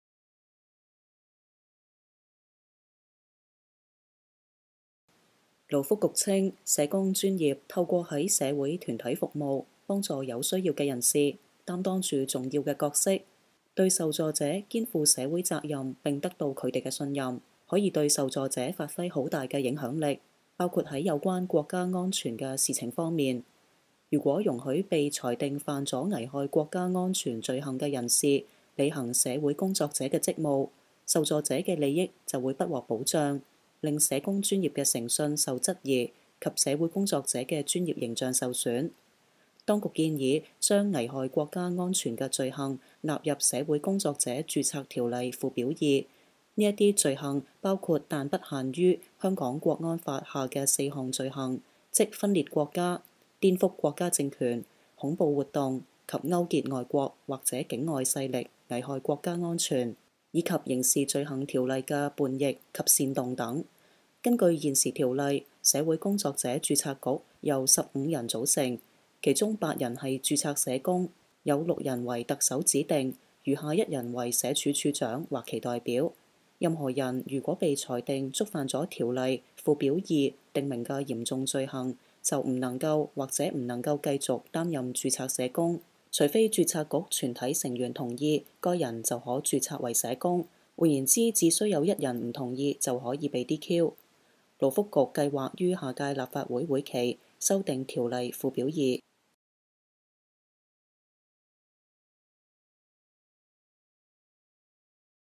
(字幕)政府擬修訂社工註冊條例 禁違國安法者擔任社工
勞工及福利局（勞福局）昨日向立法會呈交文件，建議修訂《社會工作者註冊條例》附表2，納入「危害國家安全的罪行」，被裁定觸犯危害國安的人士將不能或不能繼續擔任註冊社工。